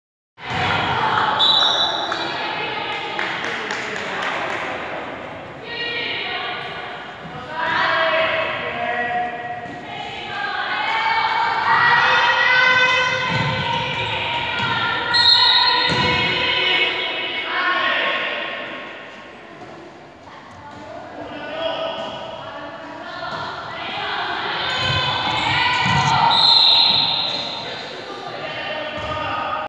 Bács-Kun Sport-TV: Jánoshalma, 2016. október 1., szombat, 15 óra. Néhány amatőr felvétel látható profik játékáról, a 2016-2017-es Bács-Kiskun megyei női kézilabda bajnokság őszi idény 3. fordulójában lejátszott Jánoshalma - Kalocsa mérkőzés 31-45. percéből